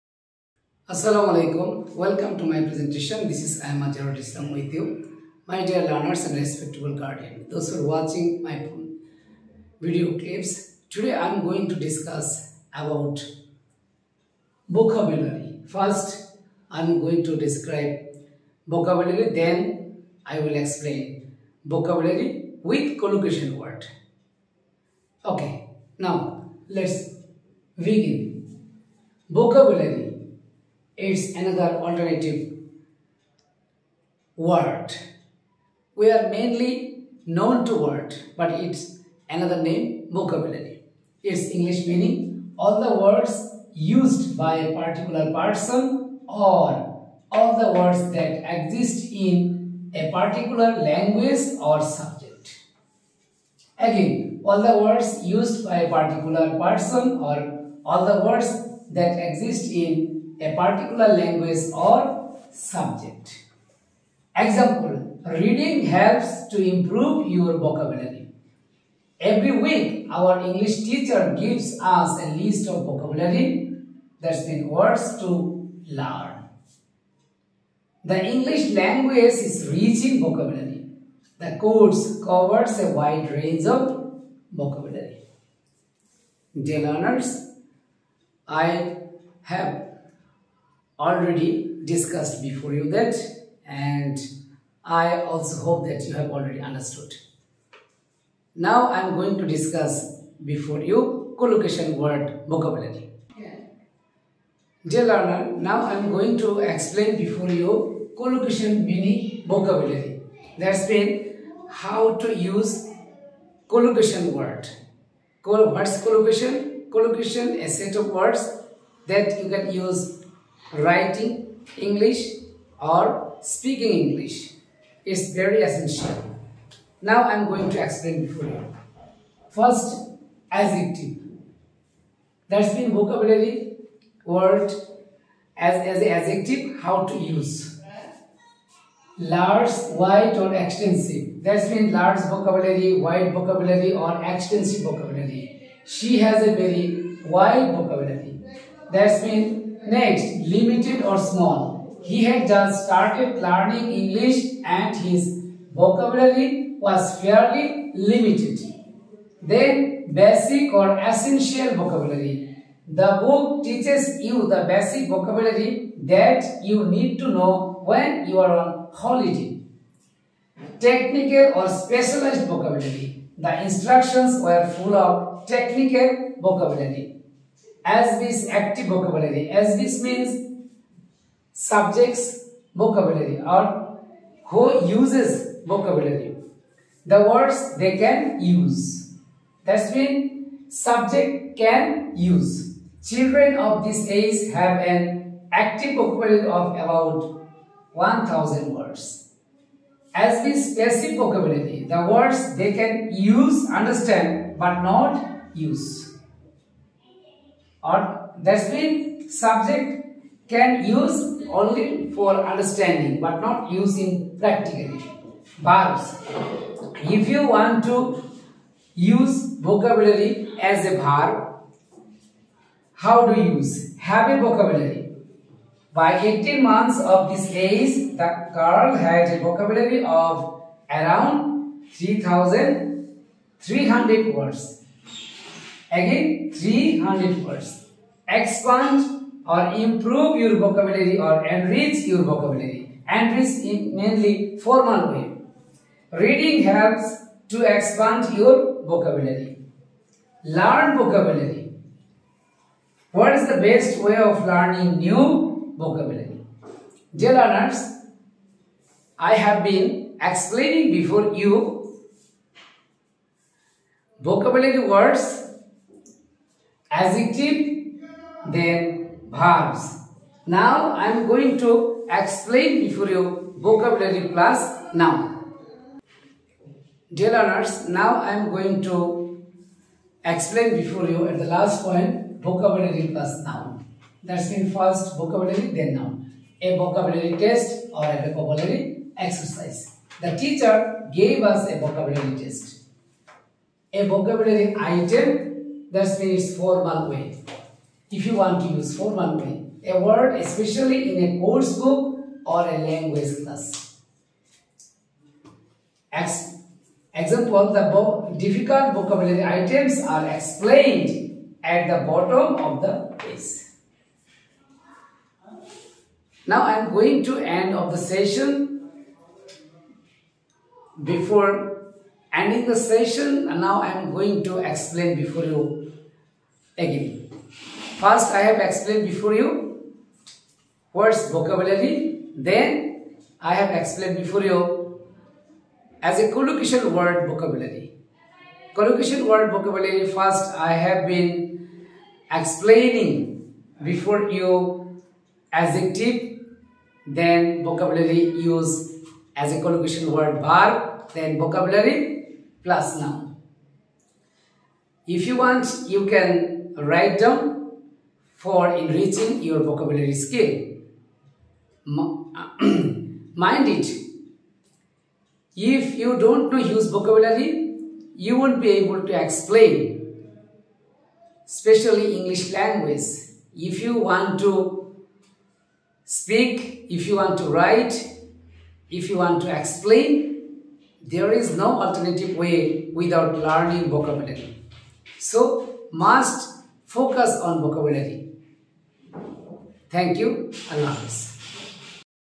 Collocation word' Vocabulary' '
I have explained the vocabulary. I also explain its collocation word using.